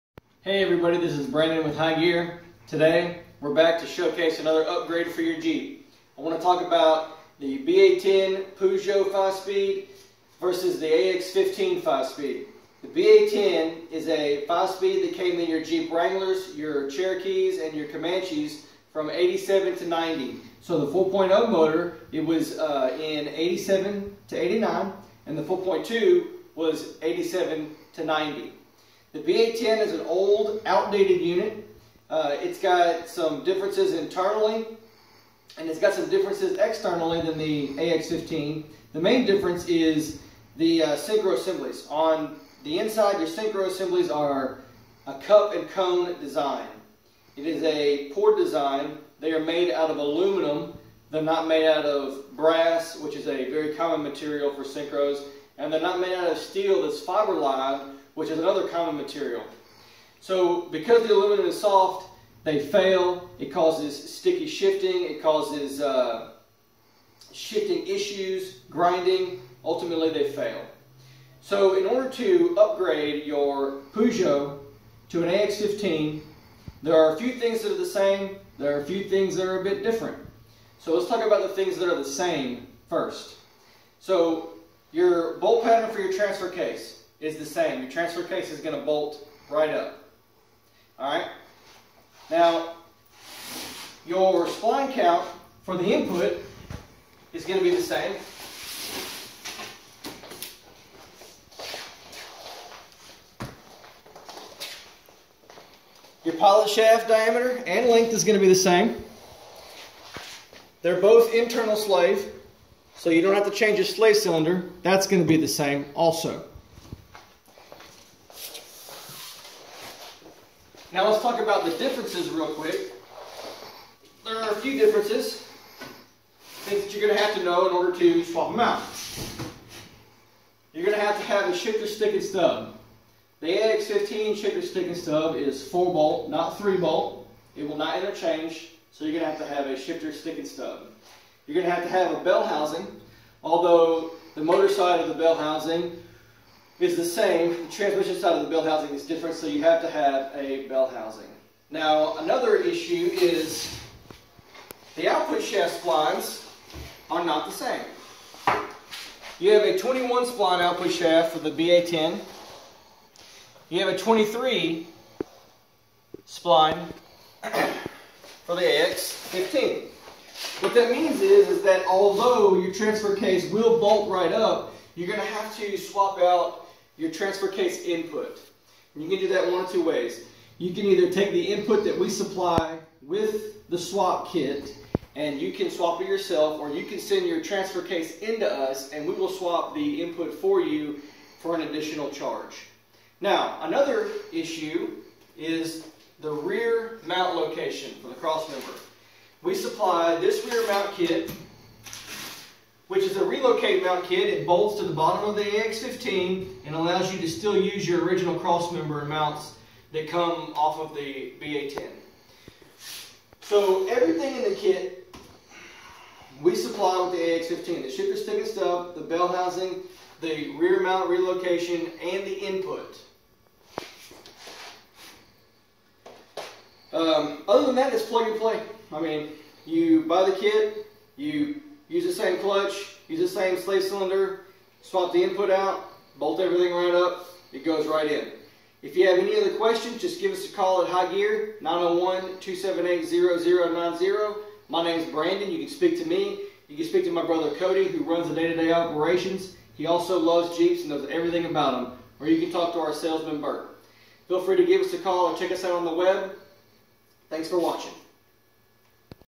Peugeot BA10 swap for AX15
The BA10 is a poorly engineered 5 speed manual transmission and can easily be swapped for an AX15 5 speed manual transmission. This is a solid upgrade for your Jeep Wrangler and other Jeeps which used the BA10. Brandon at High Gear will discuss all the details and advantages of this swap during this video.